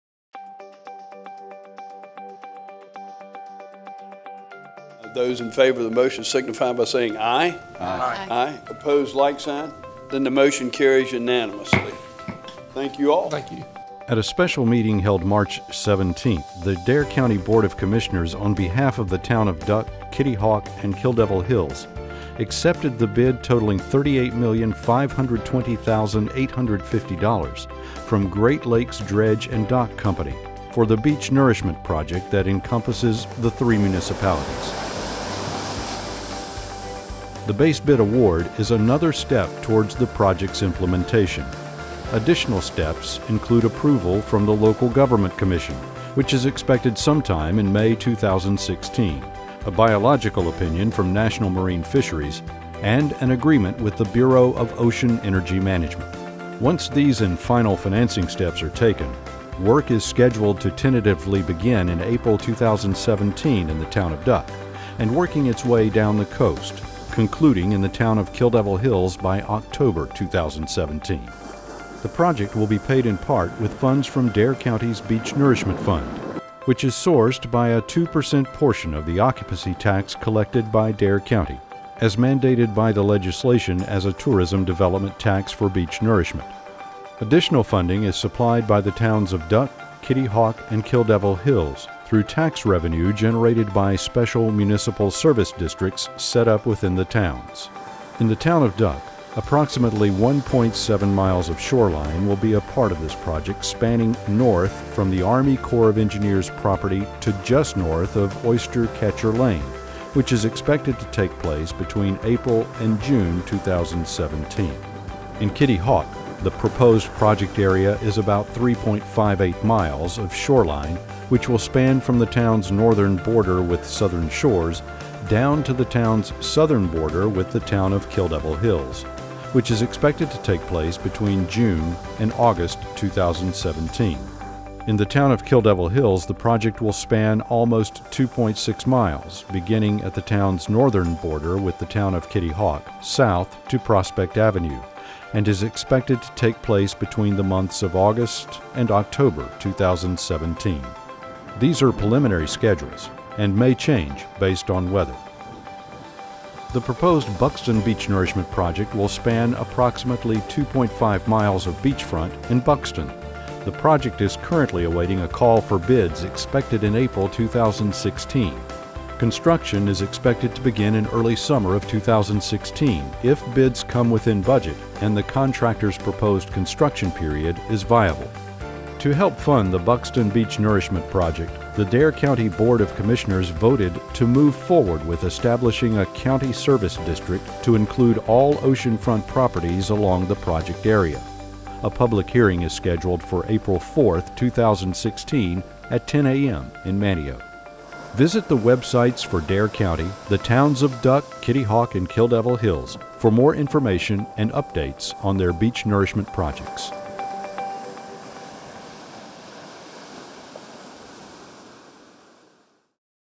March 2016 Beach Nourishment Update by Dare County Government Channel
The Dare County Government Channel provided an update on recent beach nourishment projects located in Dare County.  Visit morebeachtolove.com for an overview on projects in Dare County, for specifics on the Town of Duck's project scheduled to begin construction in April 2017.